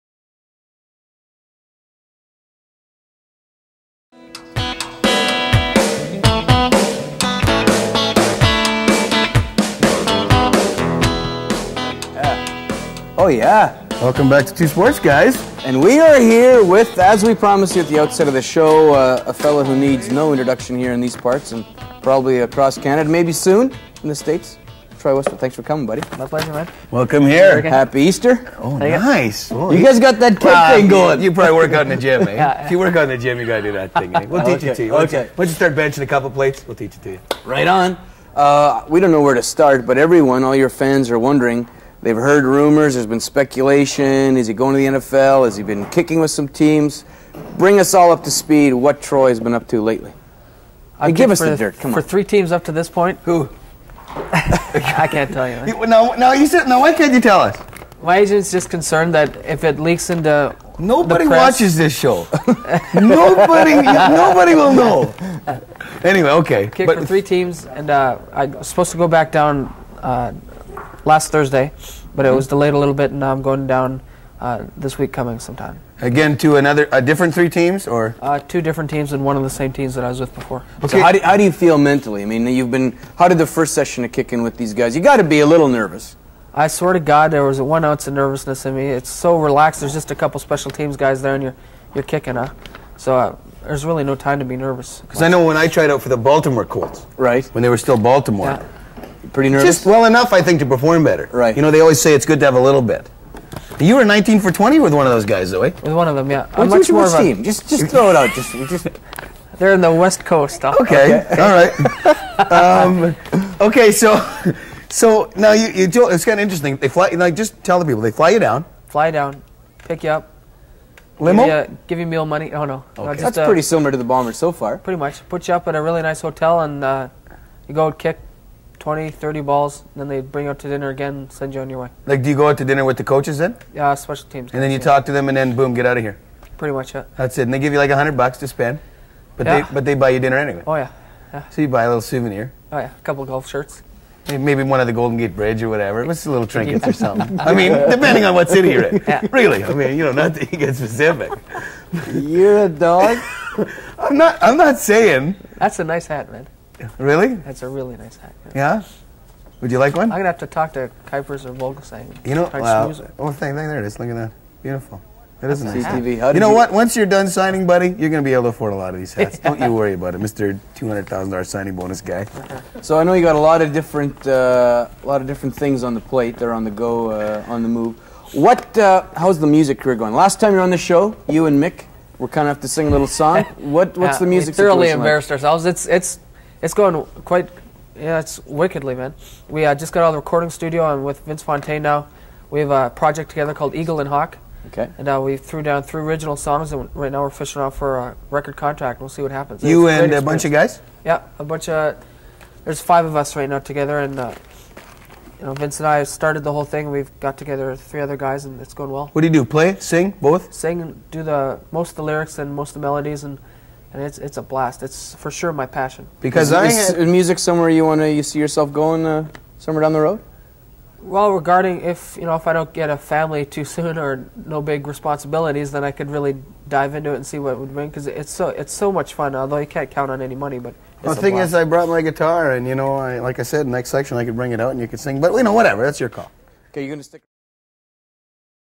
2 sports guys and troy westwood
Troy Westwood joins the 2 SPORTS GUYS to talk about his - eventually failed - trip to the States to kick for NFL teams.